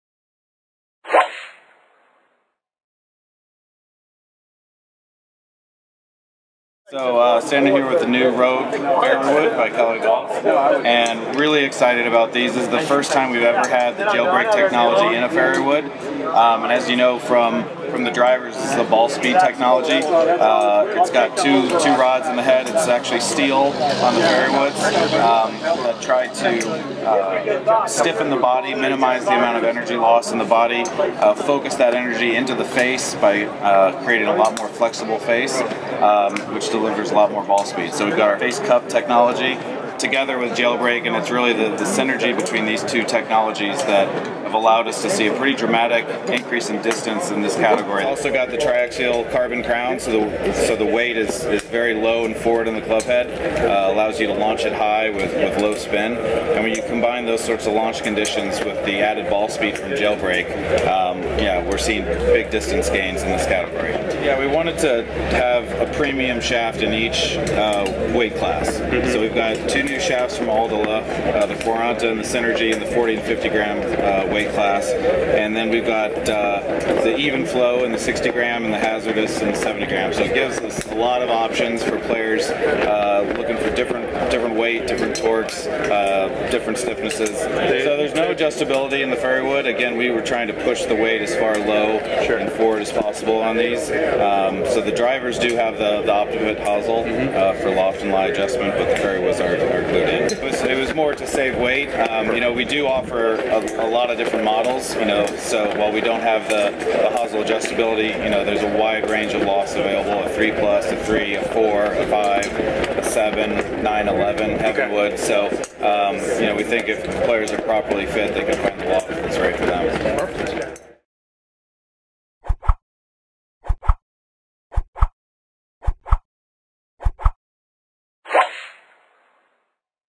Callaway Rogue Fairway Woods | 2018 PGA Merchandise Show
The GlobalGolf guys got a chance to speak with Evan Gibbs, Director of Woods R&D at Callaway Golf, about the new line of Callaway Rogue Fairway Woods.